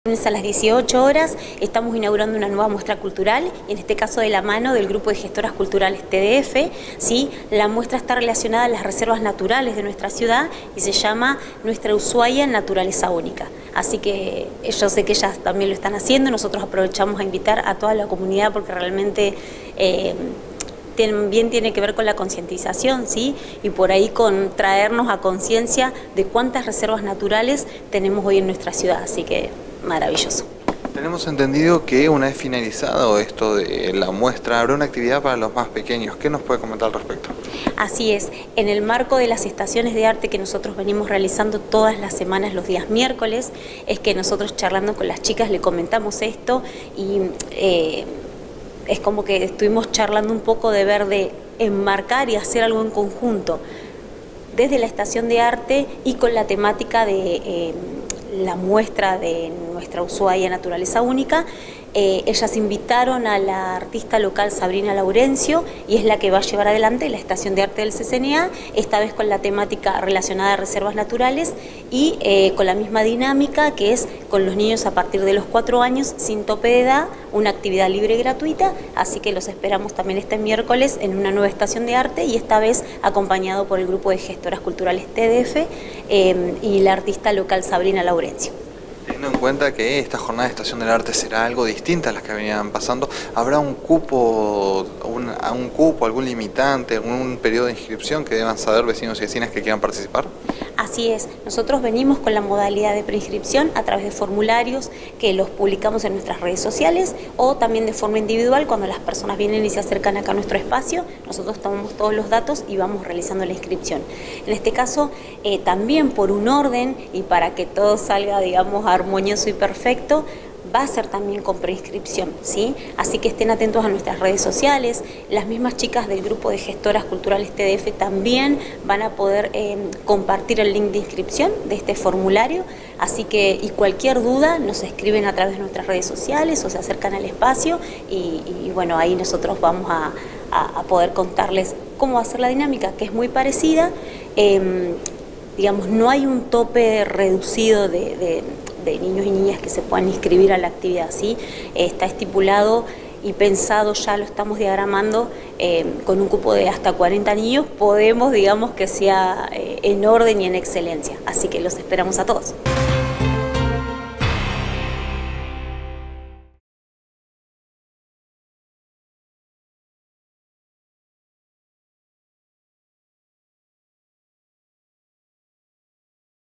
ESTACION DE PINTURA
Betiana Machado
Coordinadora del CCNA

En el marco de las actividades realizadas con Gestoras Culturales la temática de la jornada del miércoles será las reservas naturales. Esta actividad es gratuita para la comunidad solo se pide reservar su cupo debido a las dimensiones del espacio